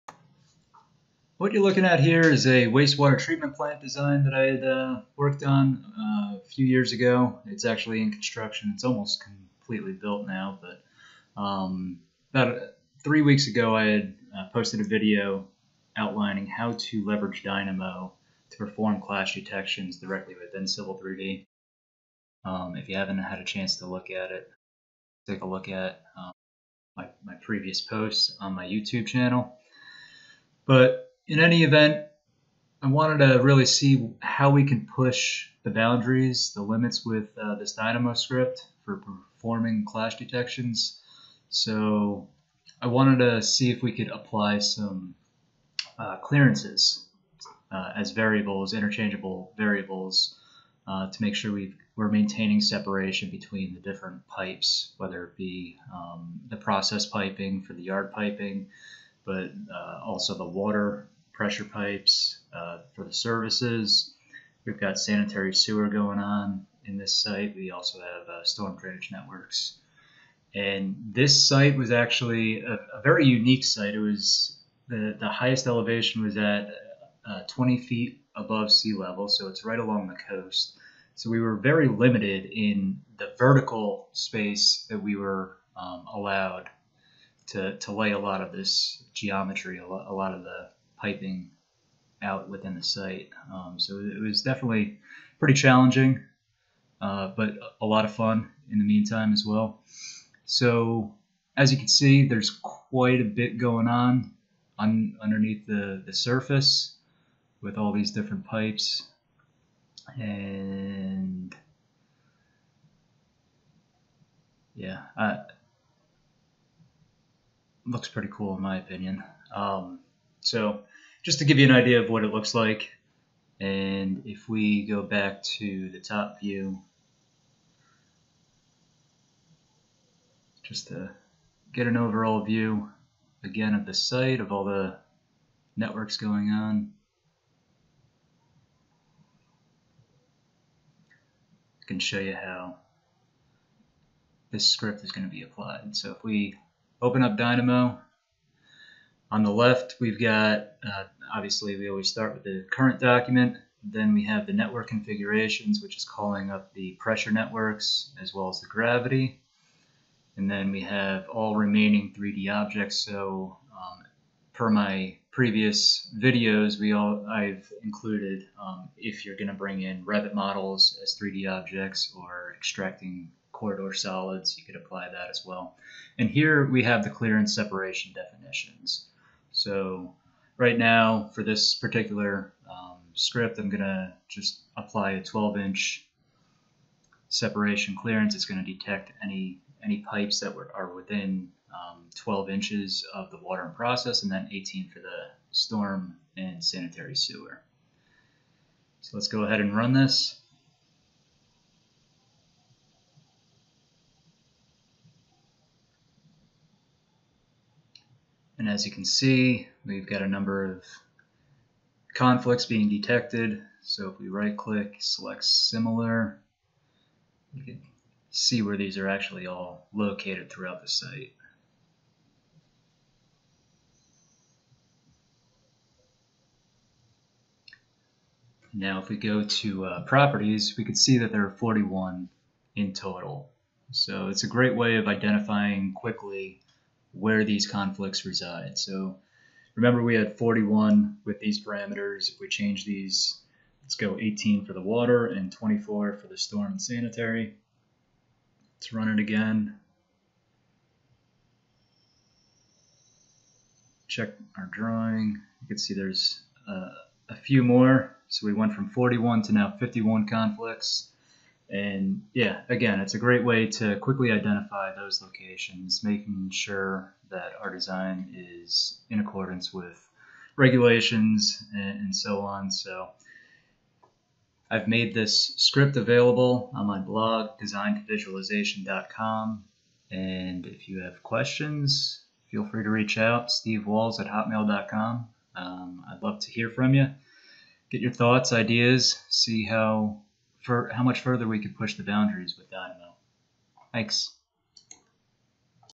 C3D Dynamo Clash Detection with Clearance Values
Continuing to push to boundaries with performing Clash Detections inside of Civil 3D by leveraging Dynamo. With the addition of adding Clearance/Separation Values, this is a perfect routine for quickly identifying conflicts in your design between Utility Networks, Revit Models, Corridors, Structural Components and any additional 3D Geometry developed inside your Civil 3D Model.